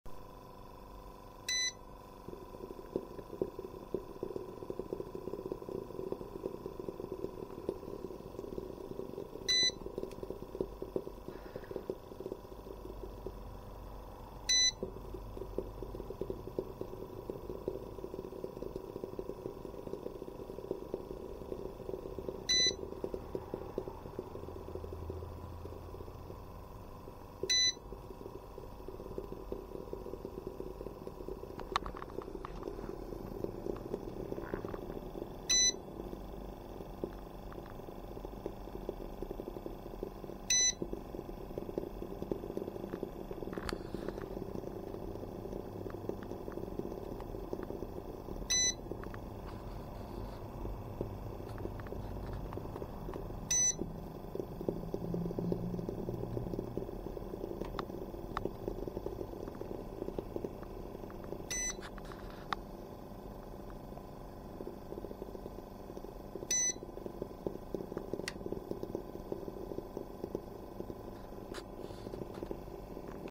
SMOKE TIMER FOR RC EXPERIMENT
Using a 3D printer element (12-18V) and switching timer. Running on only 10v-2A Switching saves battery voltage.